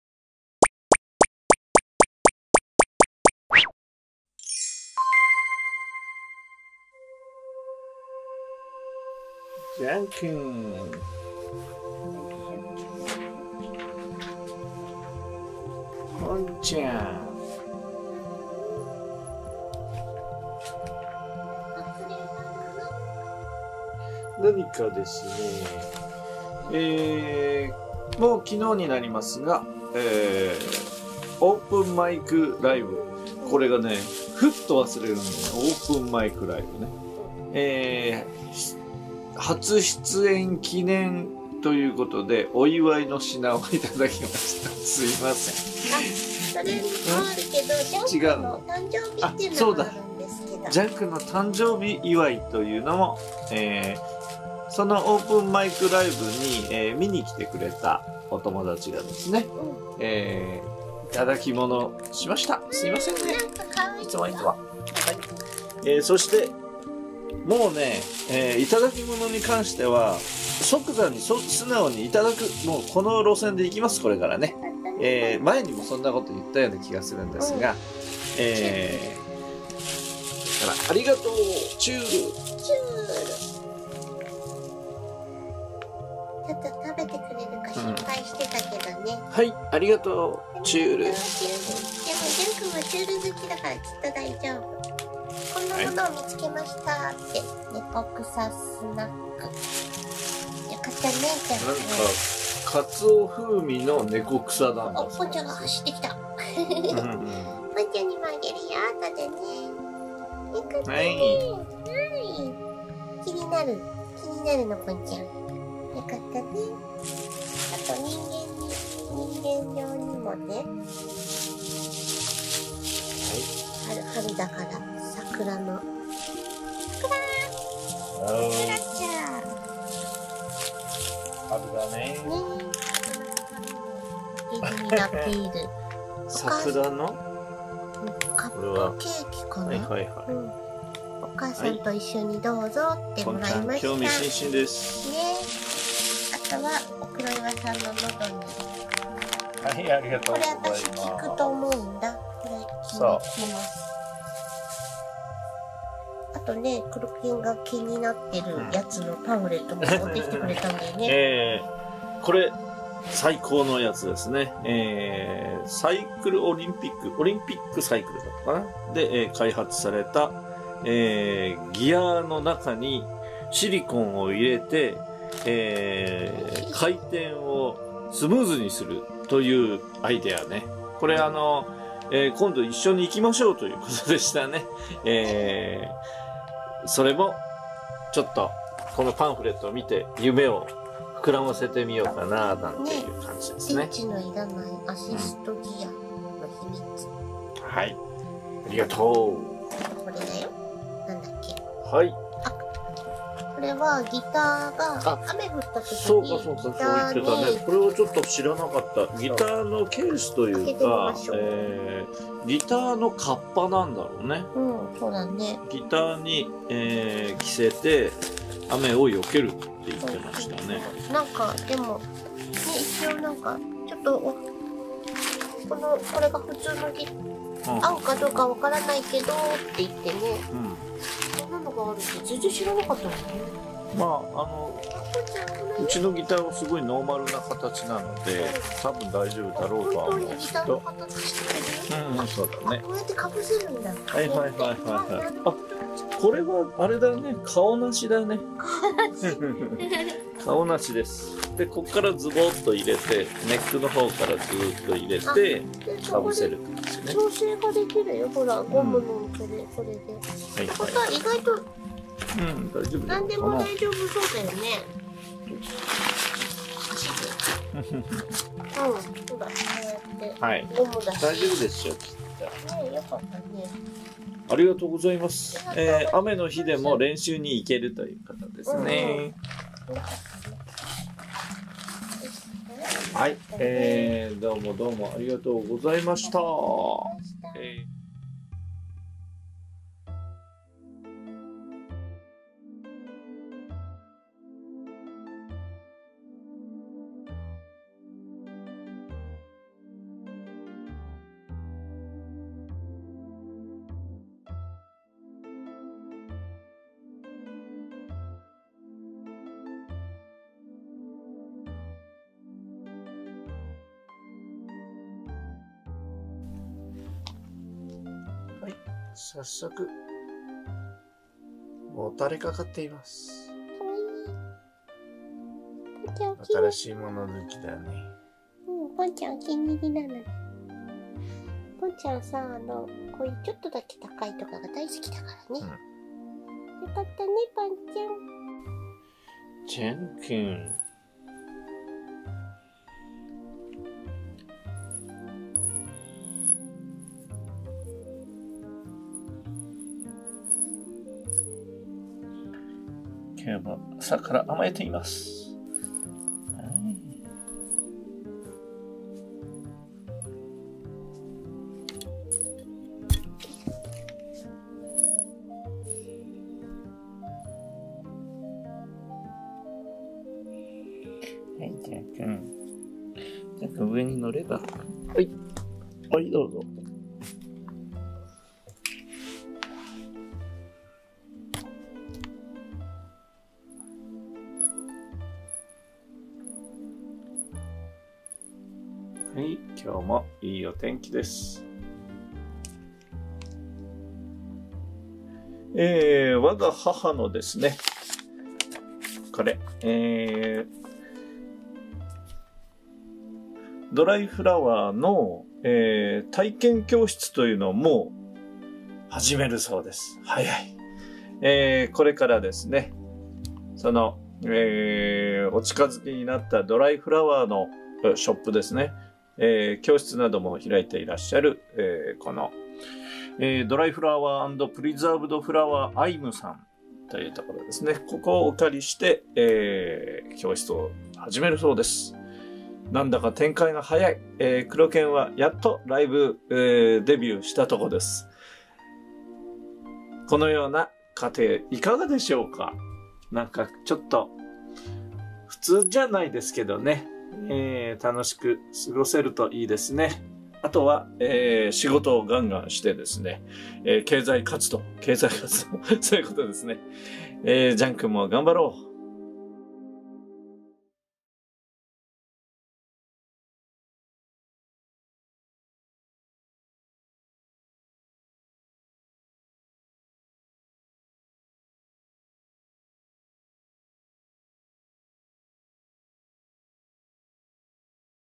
ジャンくんの誕生日プレゼントとライブデビュー祝い【Jean & Pont 1694】2019/3/31
有難や有難や、皆さんからジャンくんの９歳の誕生日のプレゼントとくろけんパパのオープンマイク・ライブのデビューのお祝いをいただきました。
わざわざH市まで出向いてライブを見ていただいた上にご祝儀をいただいて感謝です。
皆さんの好意に甘えてばかりでは申し訳ありませんが、今はできるだけ面白い動画を作ることで恩返しとさせていただきます。
また時期が来れば保護猫のためのなんらかの貢献ができればとも思います。